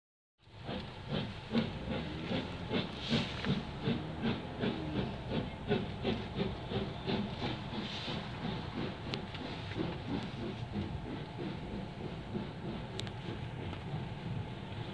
45379 leaves alton
The Mid Hants Railways Black 5 No 45379 Departes Alton With the Last train to Ropley With the Santa For Ropley.